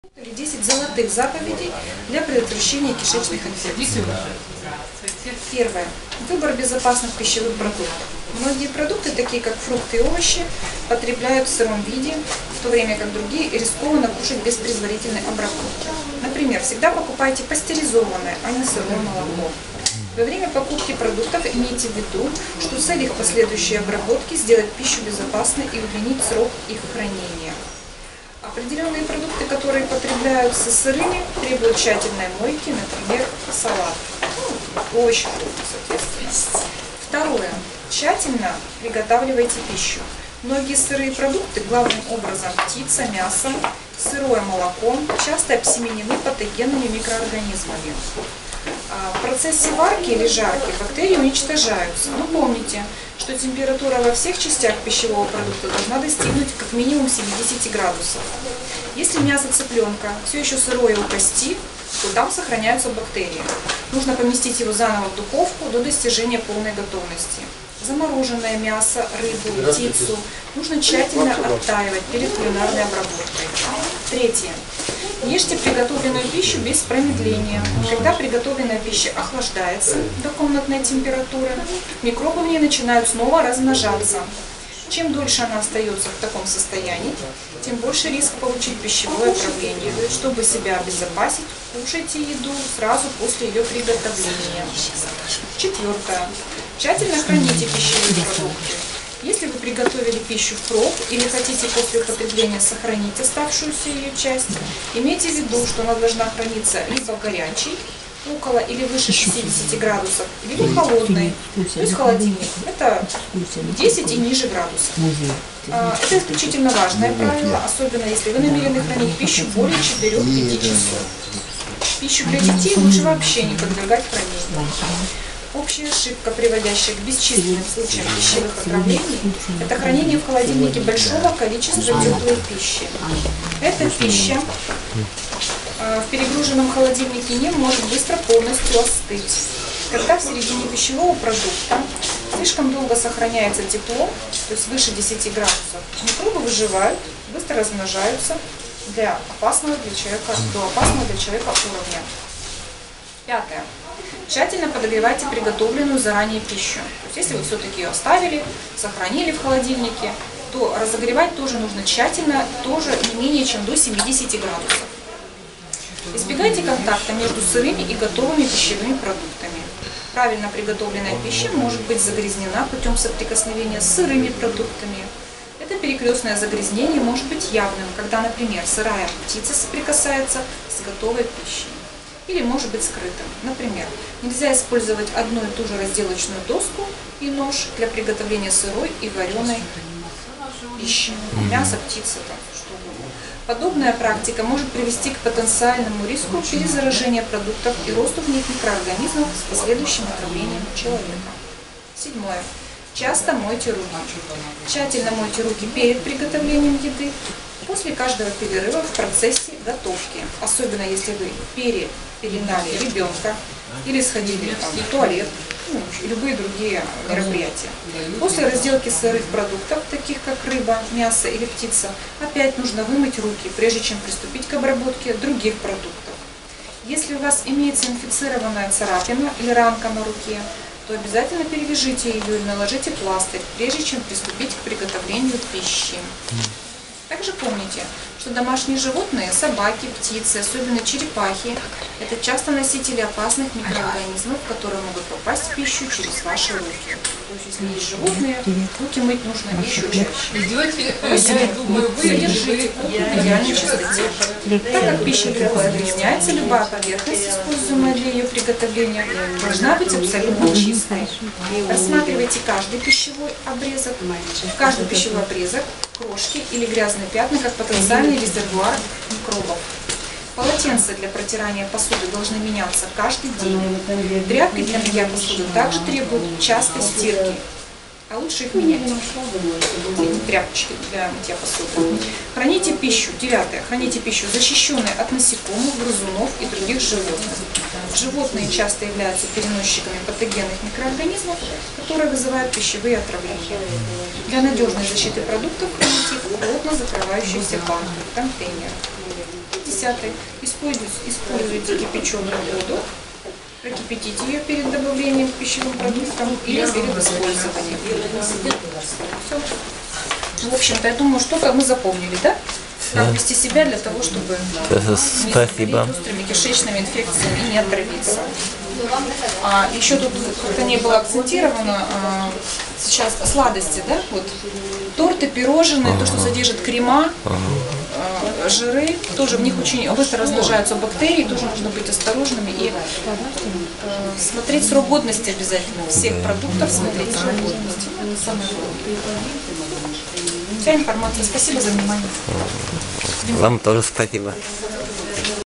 Лекция врача
Лекция врача о профилактики кишечных заболеваний.